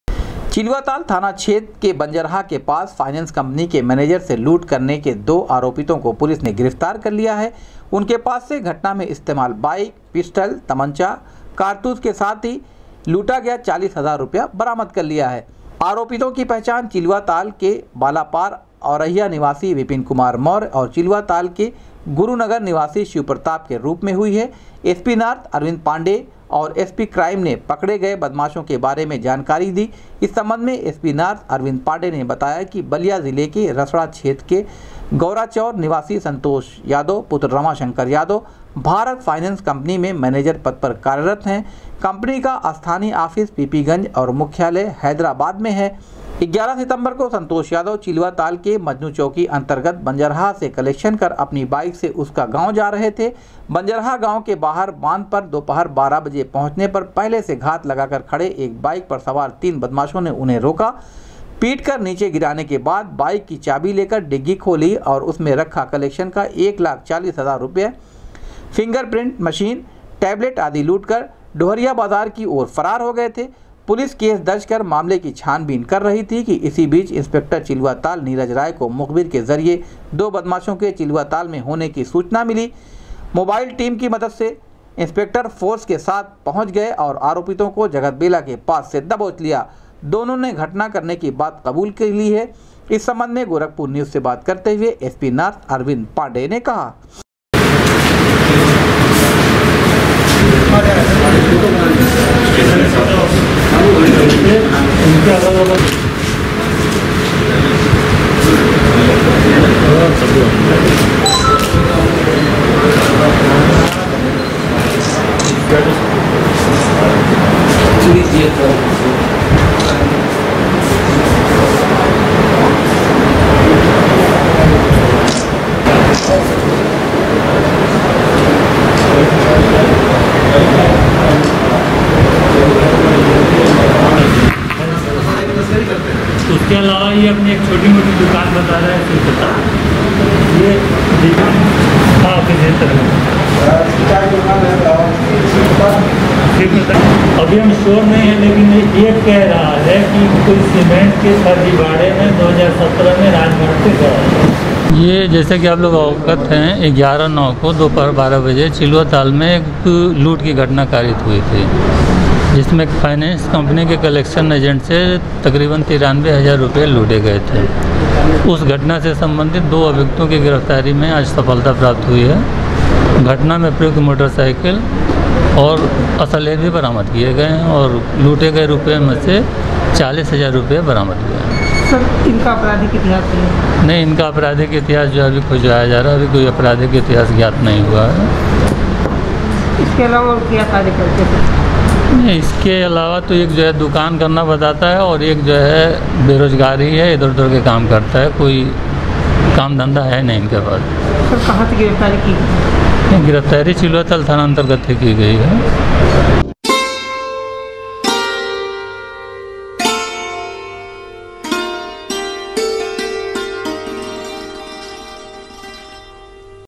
बंजरहा के पास फाइनेंस कंपनी के मैनेजर से लूट का पुलिस ने किया पर्दाफाश, दो लुटेरों को किया गिरफ्तार
बंजरहा के पास फाइनेंस कंपनी के मैनेजर से लूट का पुलिस ने किया पर्दाफाश, दो लुटेरों को किया गिरफ्तार,घटना में इस्तेमाल बाइक, पिस्टल, तमंचा, कारतूस के साथ ही लूटा गया 40 हजार रुपया किया बरामद, भेजा जेल